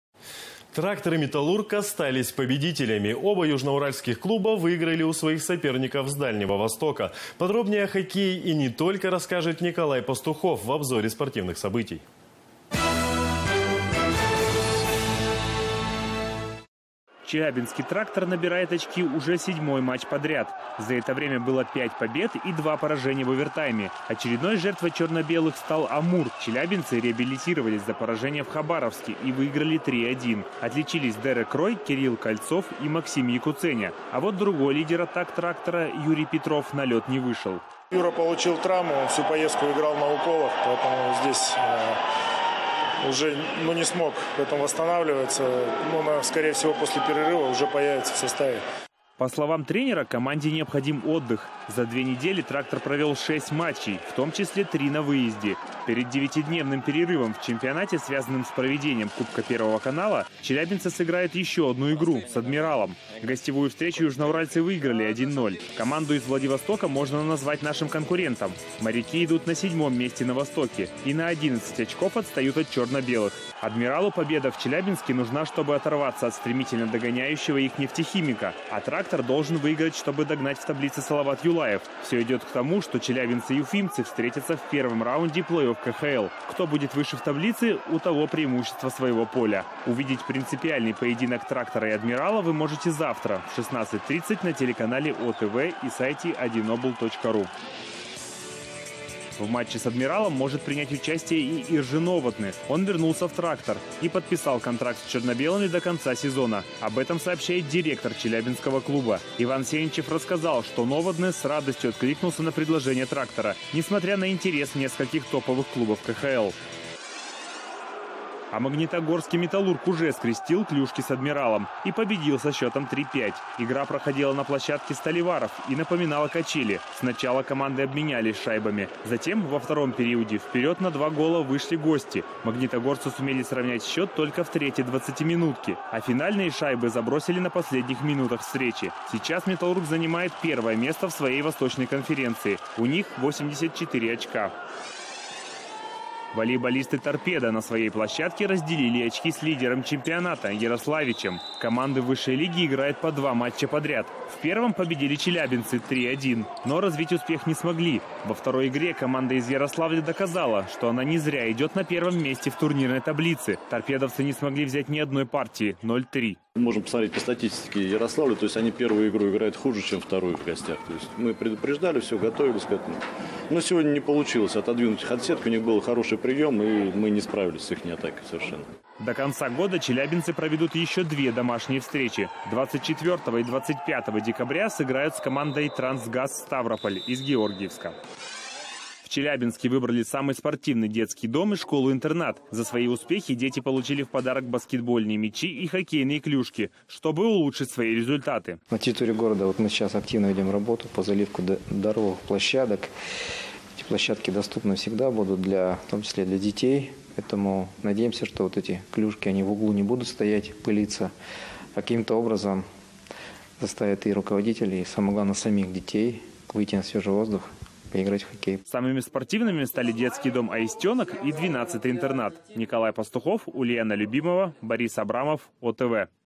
Обзор спортивных событий от 09.12.2016
Трактор и Металлург остались победителями. Оба южноуральских клуба выиграли у своих соперников с Дальнего Востока. Подробнее о хоккее и не только расскажет Николай Пастухов в обзоре спортивных событий.